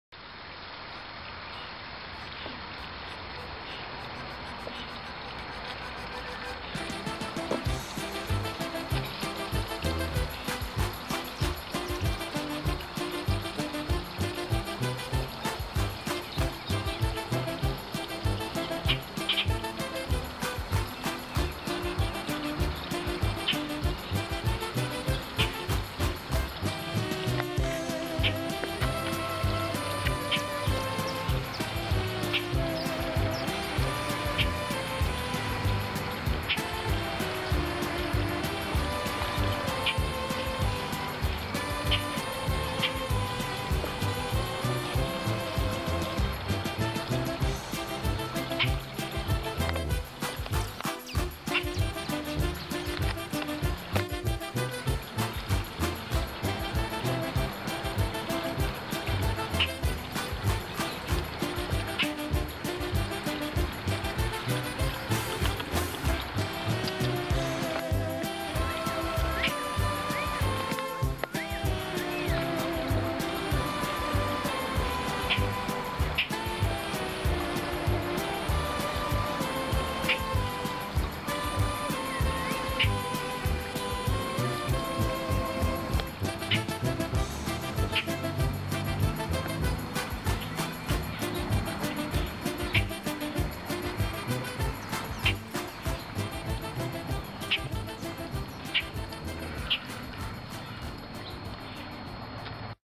Drunk Crow.wmv
I usually divide a gallon of bleach between my 2 vanity pools as it is cheaper than algeacide and keeps the water crystal clear and free of mosquitos. I poured a bottle of old vodka in one of the pools so that I could return the glass to the recycle bin...next morning, I woke to the sounds of Mrs. Robin bitching to her husband, Mr.Crow, who was  PLASTERED and sitting and "singing" on one of the pond lights.  He was too drunk to fly and/or care that I was getting some good video of this domestic dispute.  He subsequently stumbled off into the grasses.